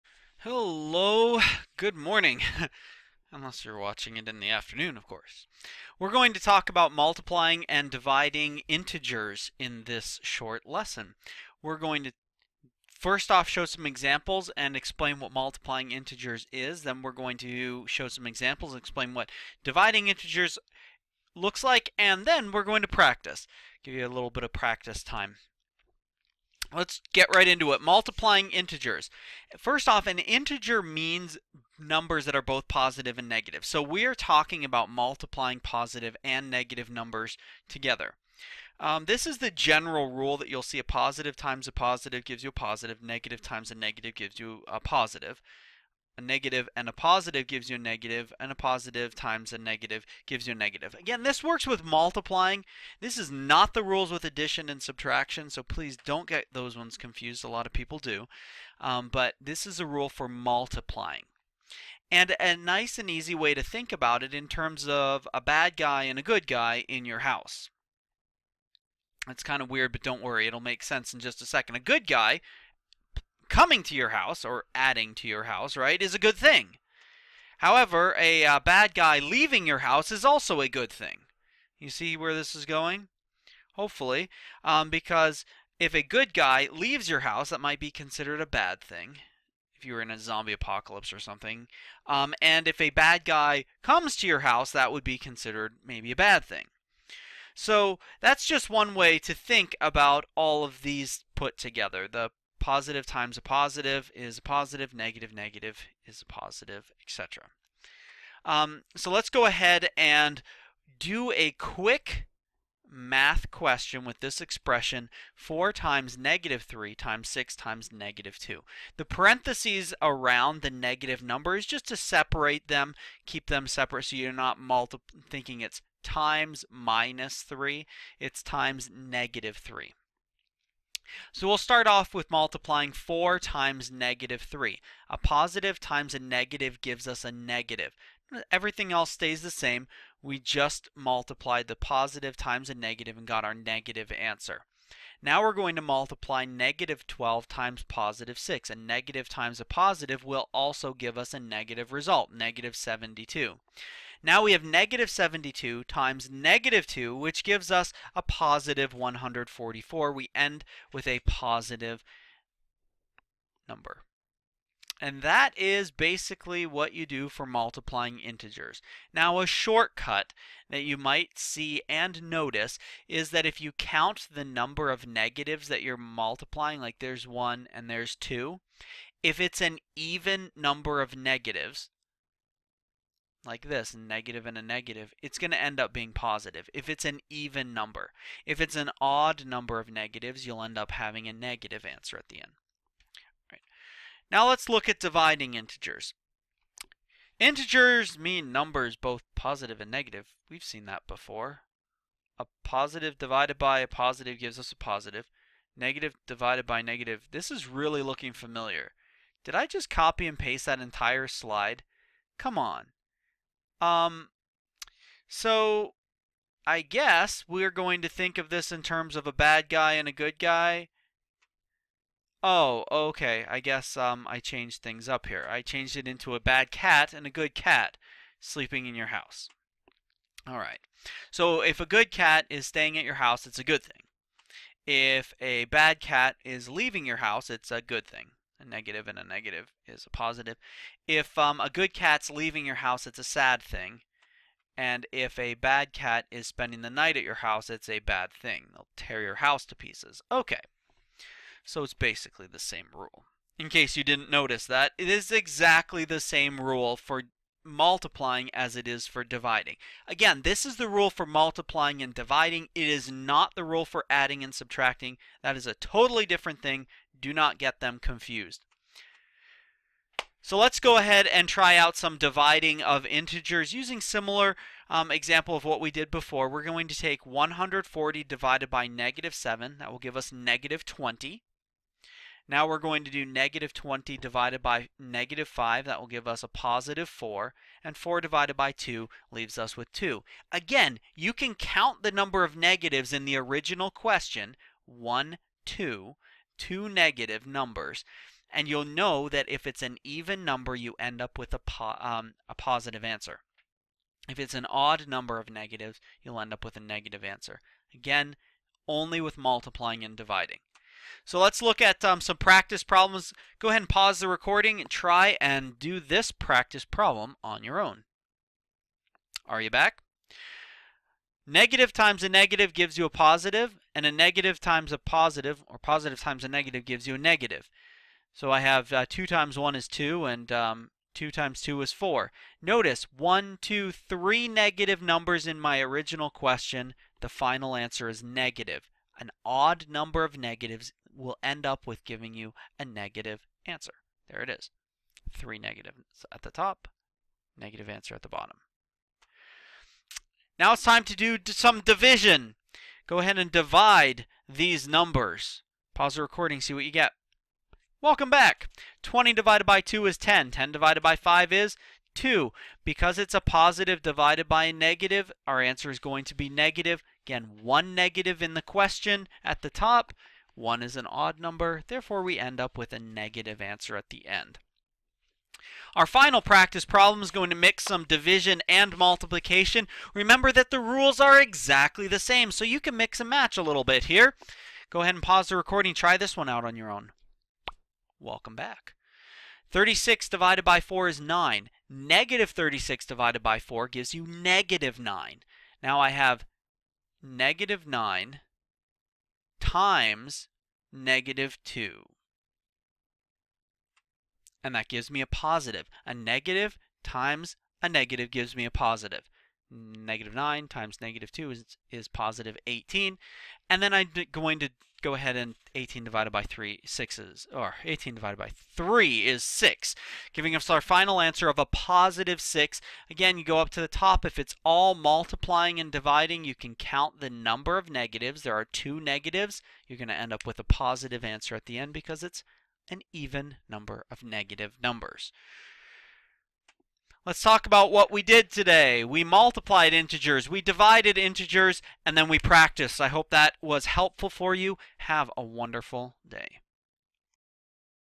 Multiply and Divide Integers (Simplifying Math)
This lessons shows some tips and tricks for multiplying positive and negative numbers.